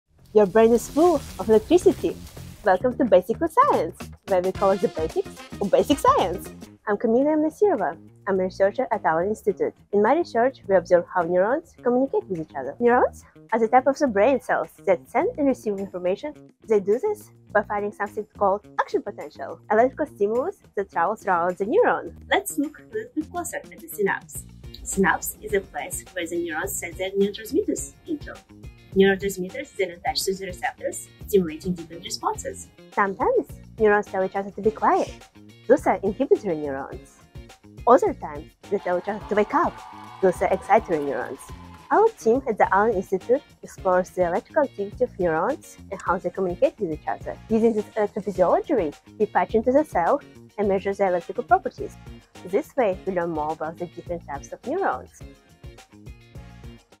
How do neurons talk to each other? | Basically Science
Lights, camera, action potential! 🧠⚡️

In our latest BasicallyScience, we're exploring how neurons talk to each other using their own chemical language and how we can eavesdrop on them. 👀